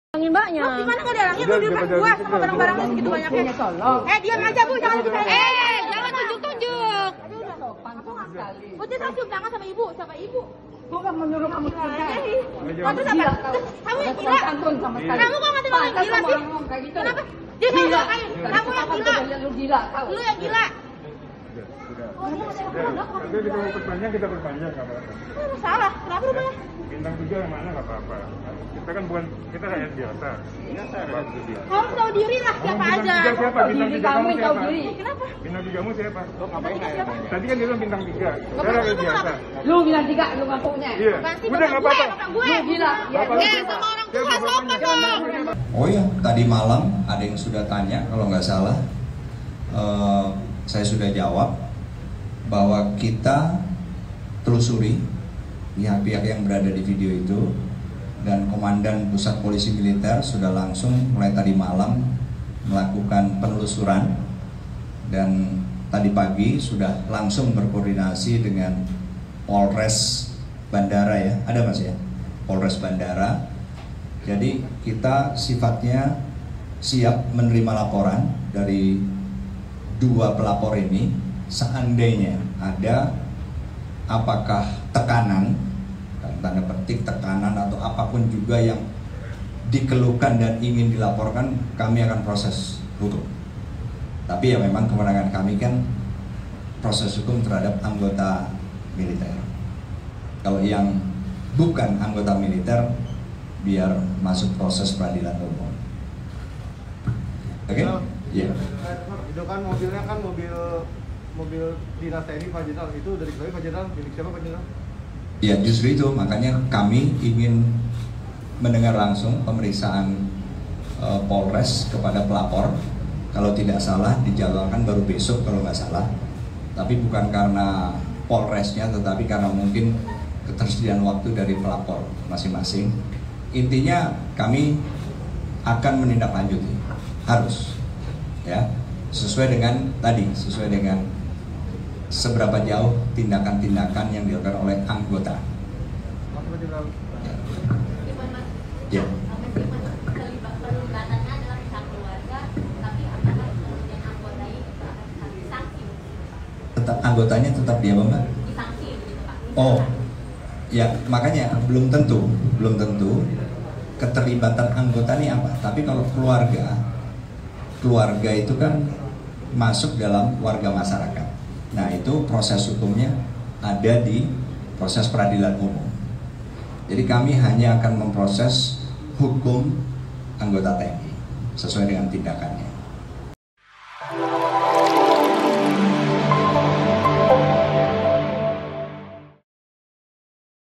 SOSOK Anggita Pasaribu, Sebut Jenderal Bintang Tiga hingga Kenal dengan Megawati Soekarnoputri
TRIBUN-MEDAN.COM - Ketua DPR DKI Jakarta Prasetyo Edi Marsudi akhirnya membuka identitas perwira tinggi TNI yang menelponnya untuk damai dengan Anggota DPR RI Arteria Dahlan.

Prasetyo menyebut kalau perwira tinggi itu tidak berpangkat bintang tiga, melainkan bintang satu.

Ia adalah Brigjen TNI M Zamroni.

Dilansir dari Tribun Jateng, Brigjen M Zamroni pernah menjabat Dandim Jakpus pada 2017.

Sementara perempuan yang membentak ibu Arteria Dahlan adalah Anggita Pasaribu, istri dari Brigjen TNI M Zahroni.

Tidak diketahui pasti orang yang diklaim jenderal bintang tiga oleh Anggita Pasaribu adalah suaminya atau ada pihak lain.